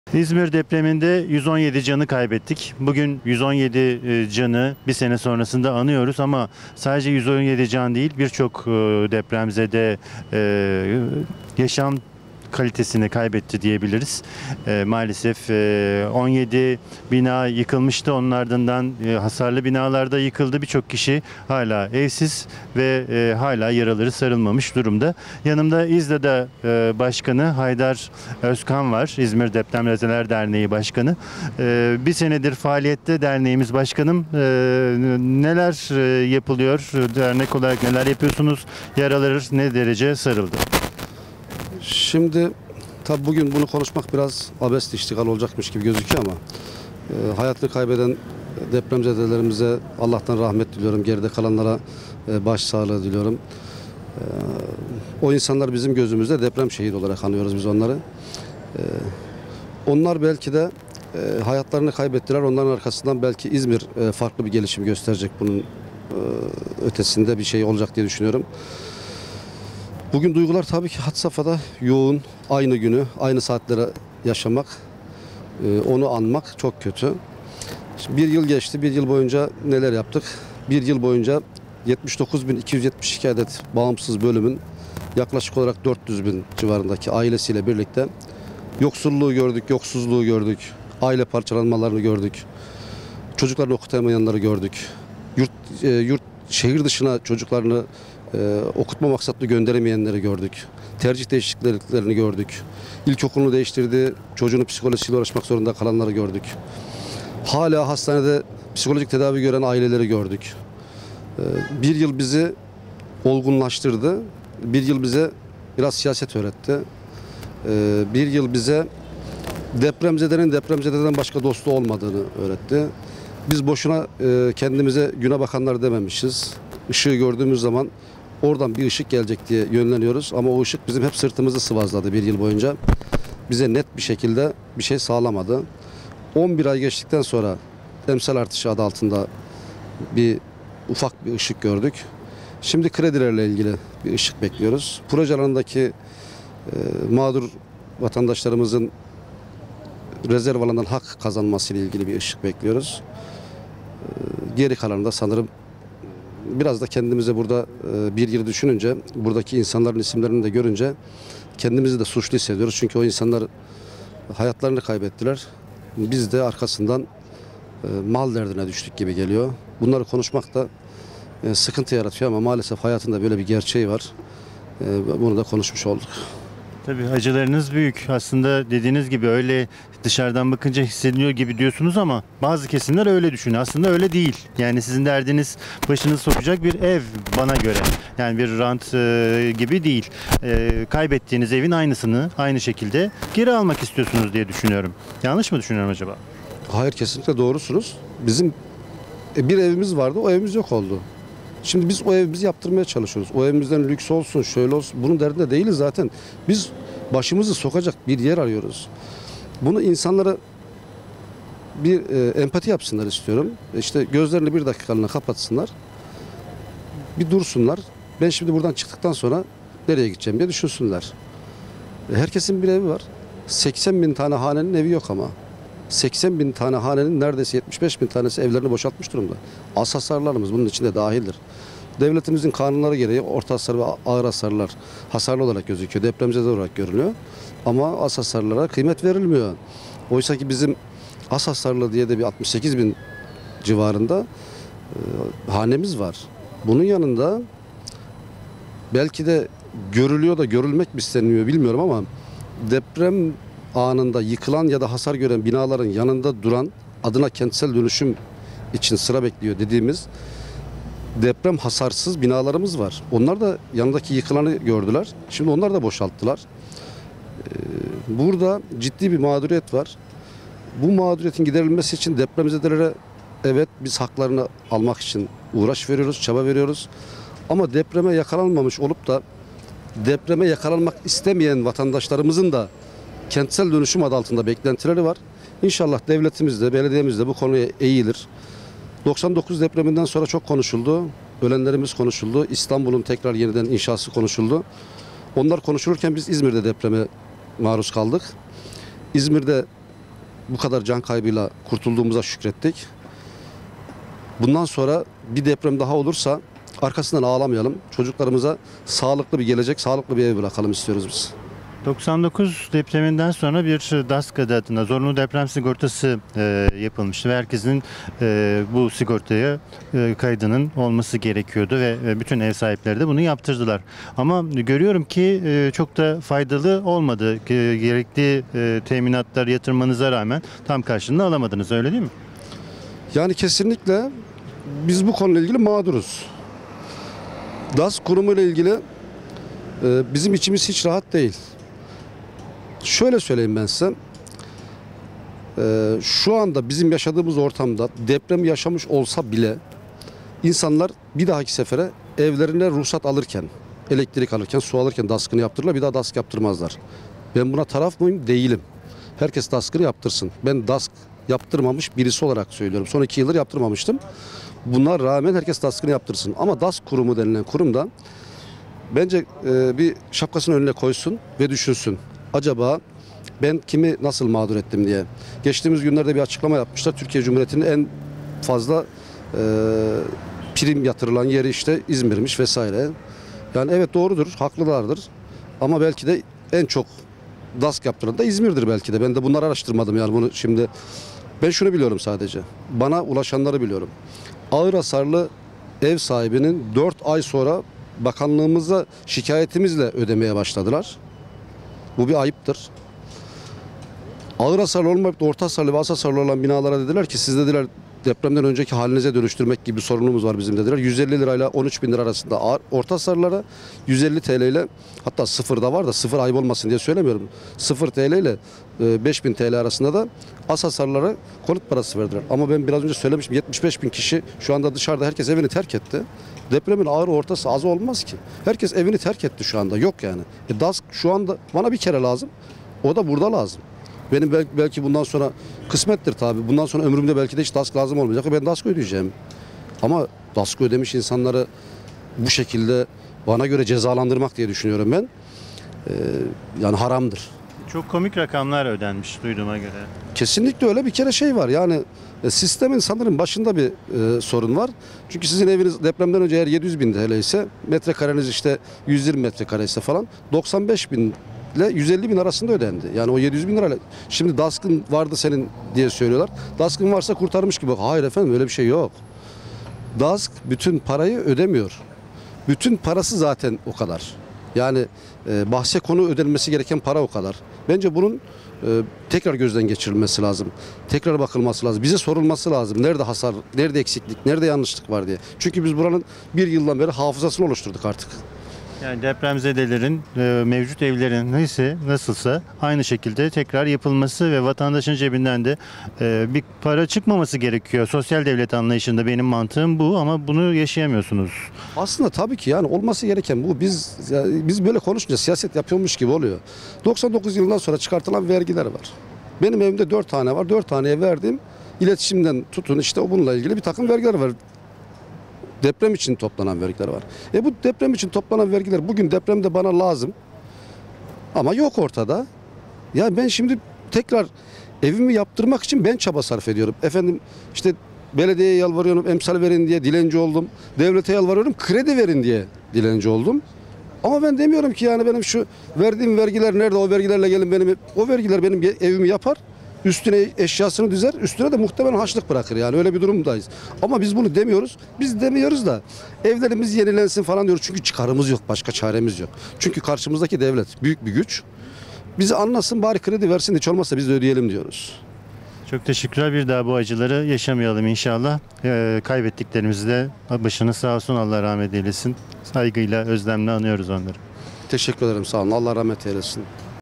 İzmir depremi unutmadı! | İZDEDA BAŞKANI HAYDAR ÖZKAN
İzmir Büyükşehir Belediyesi, 30 Ekim İzmir depreminin yıl dönümünde hayatını kaybeden 117 kişinin anısına kapsamlı bir anma programı hazırladı. Bayraklı’da deprem parkı olarak yenilenen Hasan Ali Yücel Parkı’nda Deprem Anıtı açıldı.